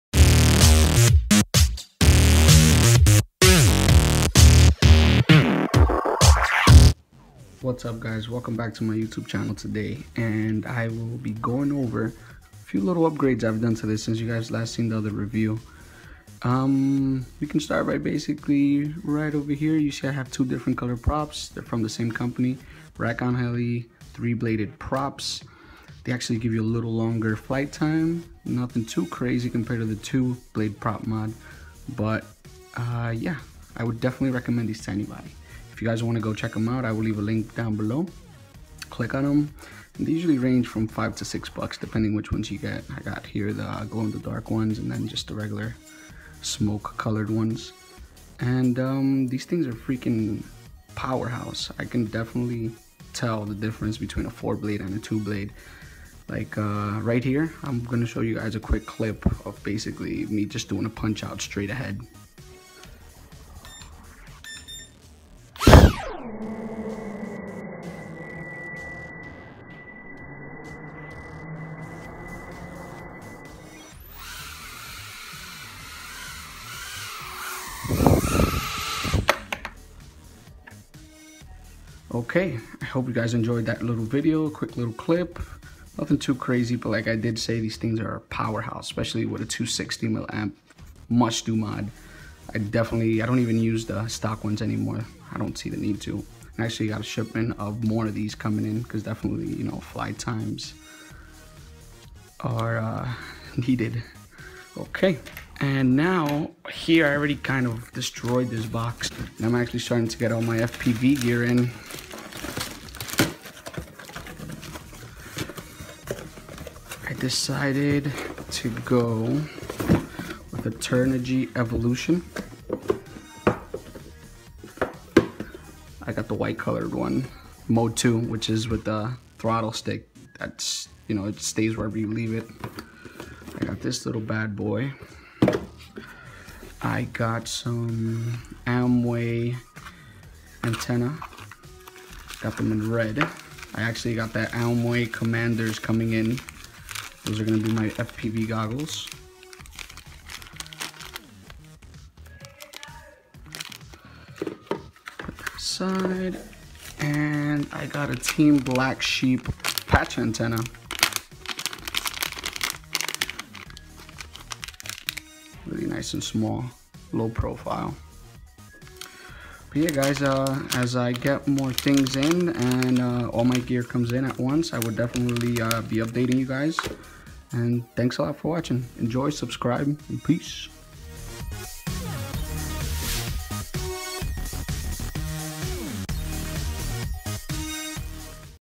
Furibee F36 Update & Quick Unboxing
Just a quick update on my furibee f36 running on Super Insane 19600kv motors and Rakonheli 3 blade props, Also a quick unboxing of my Fpv gear as it finally comes in!